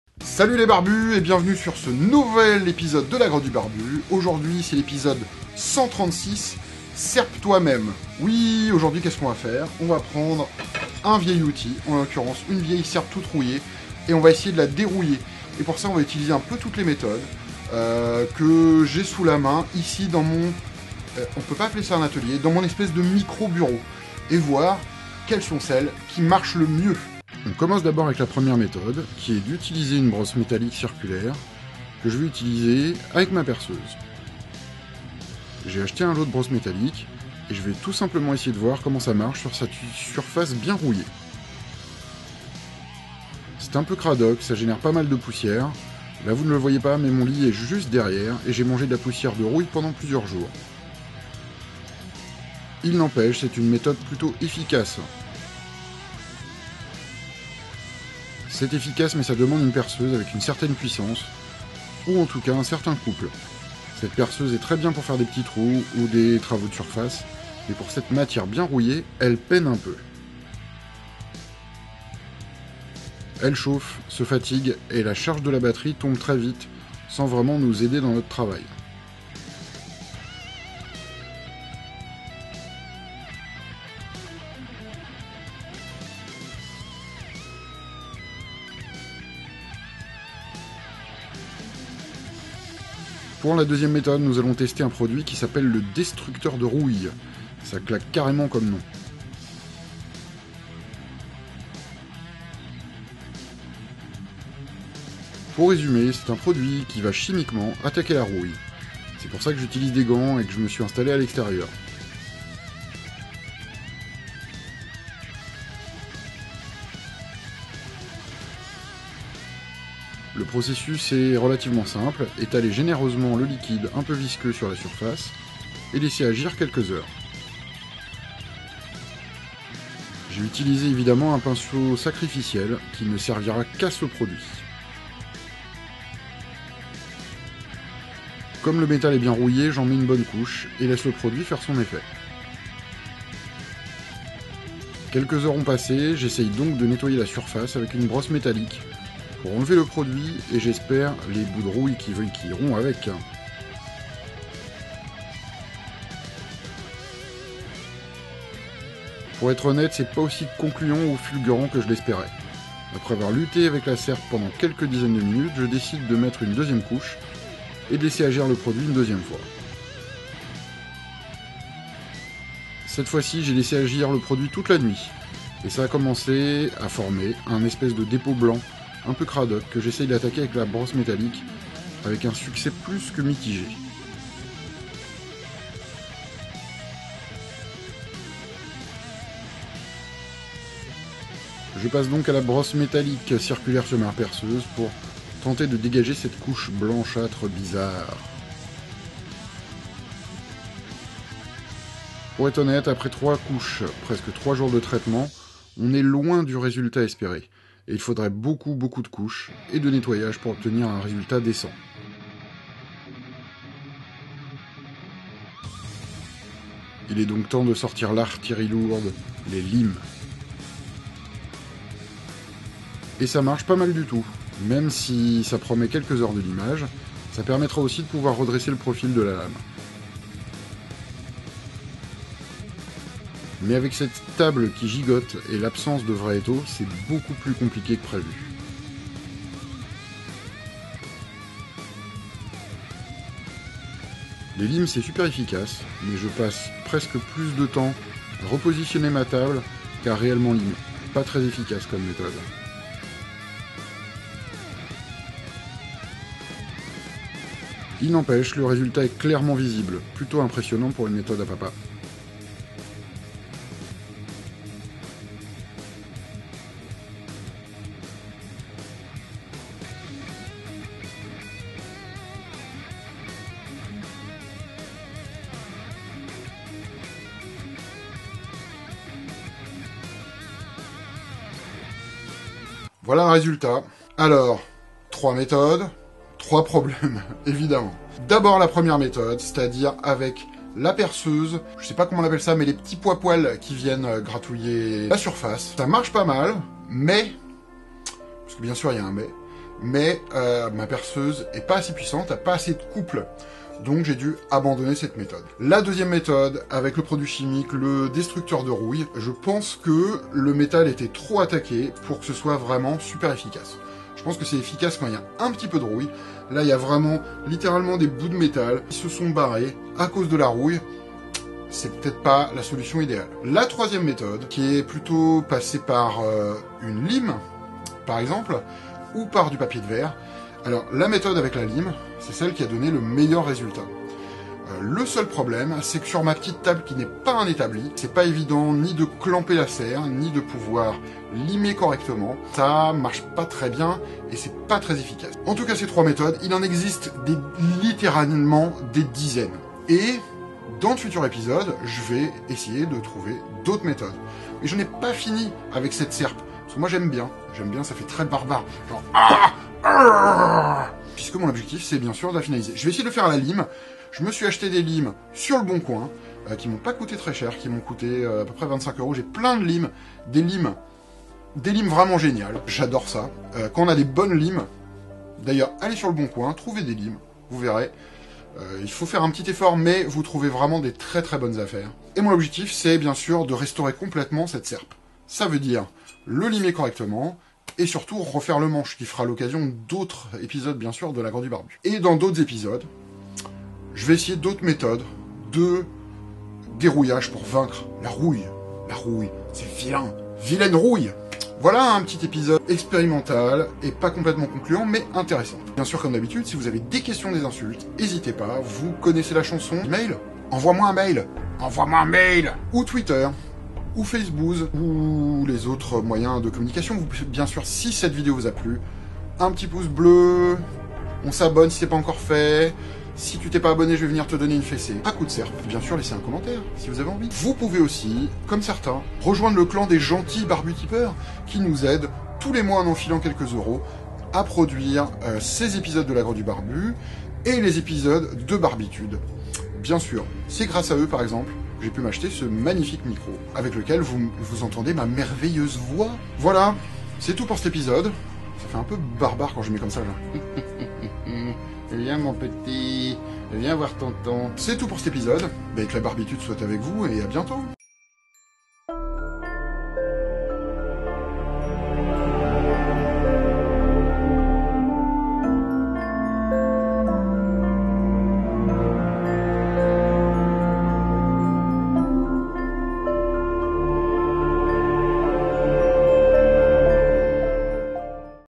SerpeToiMeme - Restauration et dé-rouillage d'un outil ancien - Ep136
Dans cet épisode je vais tenter trois méthode pour essayer d'enlever la rouille de cette vieille serpe que j'ai acheté dans une brocante.